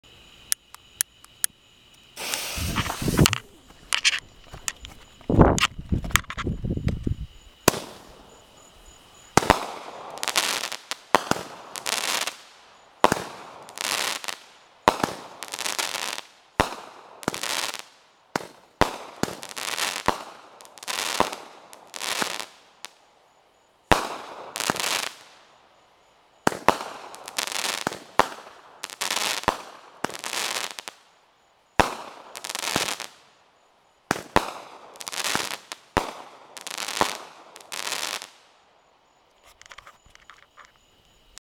Wildfire 16 Shot Firework
I always get a few of these for my show, good colors, good to get the show warmed up.